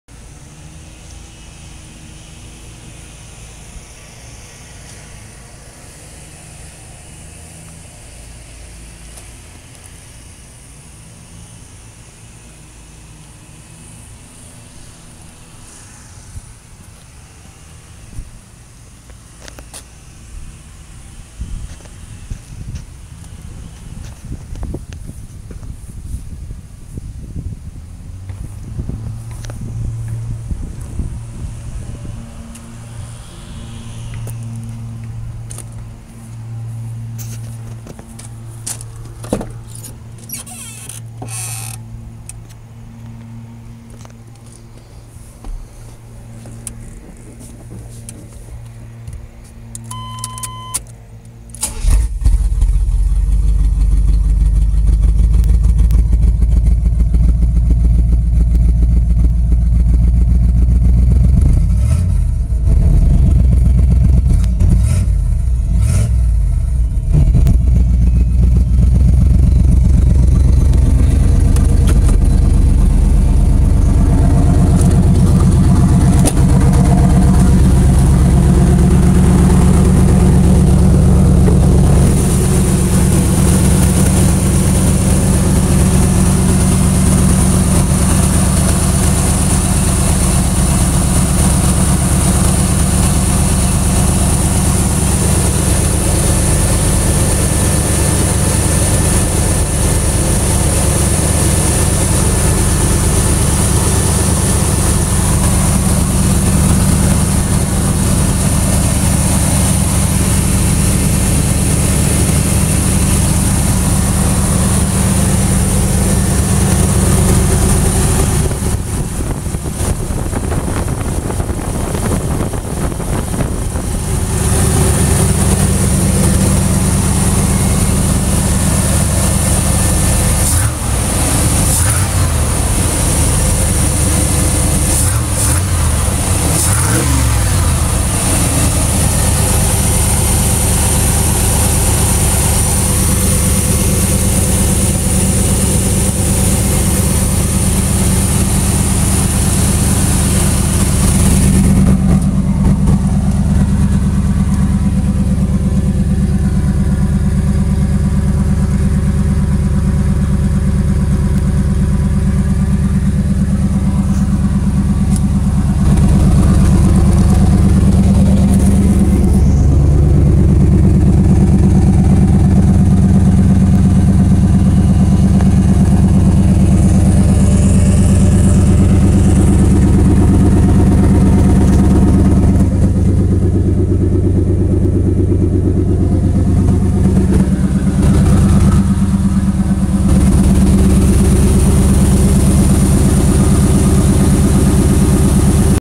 Carb swap 1992 Chevy K1500
My setup of a tbi to carburetor conversion. Holley 4160 600 cfm carb, Holley Street Dominator intake, GM HEI distributor, Holley 12-803BP fuel pressure regulator, Holley throttle position sensor kit.